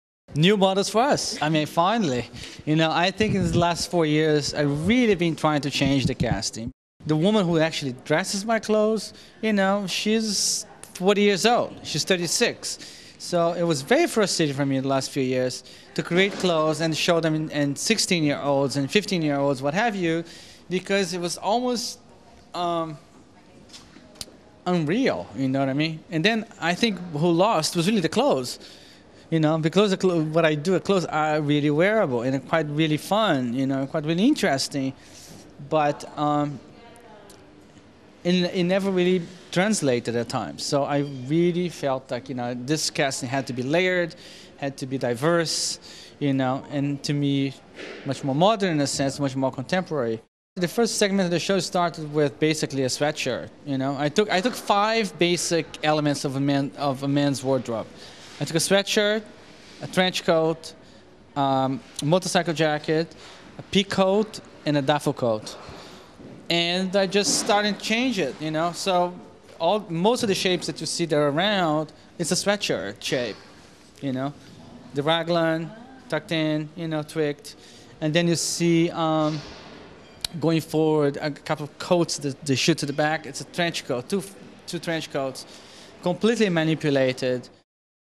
Interview with Francisco Costa backstage at the Calvin Klein Women's Fashion Show in New York
Francisco Costa, the artistic director of the women's line at Calvin Klein, talks about his latest collection based on new volumes!